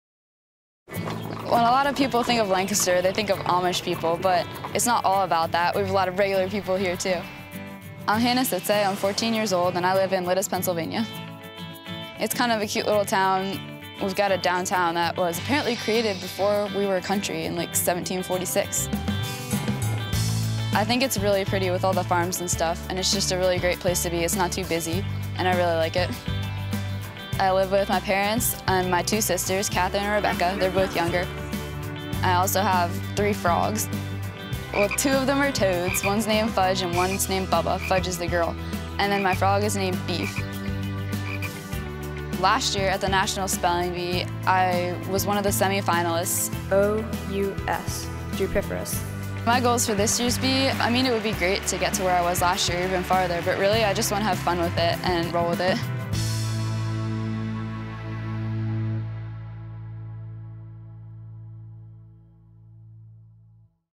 Hannah Citsay, 2014 Speller 208, from Lititz, Pa
Meet Hannah Citsay, 2014 Speller 208, who resides in Lititz, Pa.